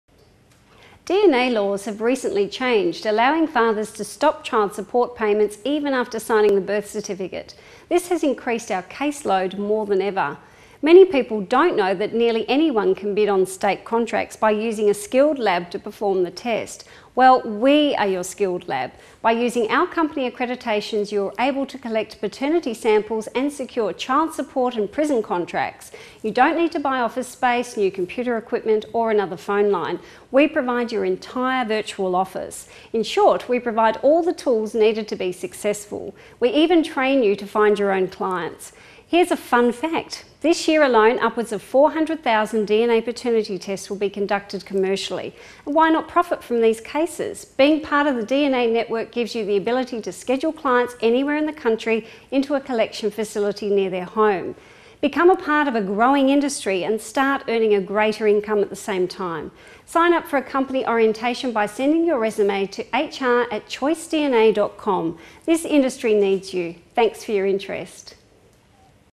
Why we are Hiring DNA Collectors?
It pays to become a partner with Choice DNA. Recent government laws has increased case our loads. Child support cases, prison exoneration, immigration and so many other DNA testing concerns.